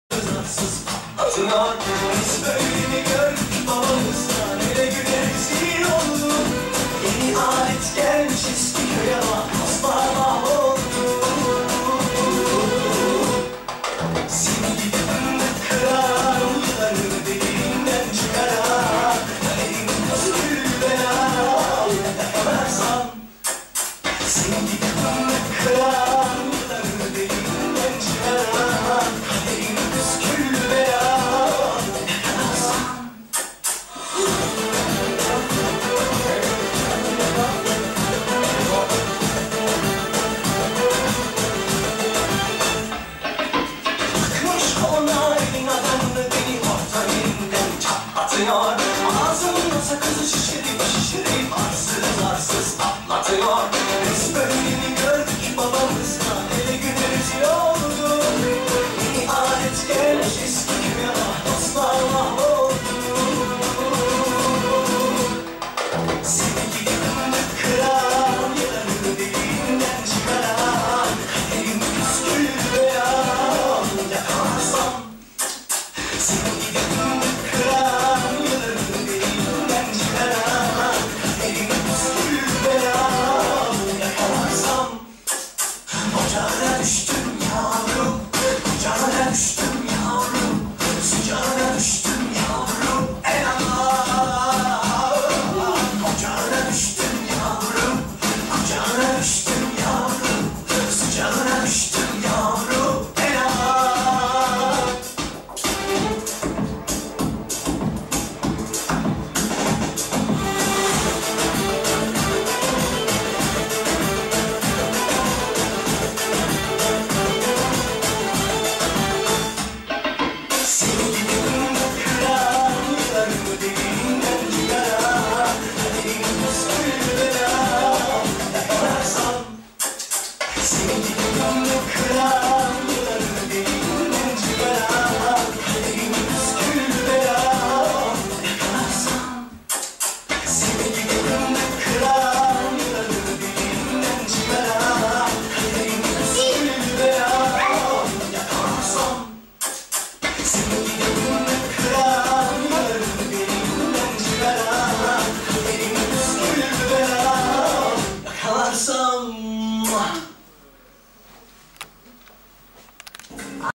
alessia
notre petite alessia qui a 5 ans fais de la danse orientale
super  ma puce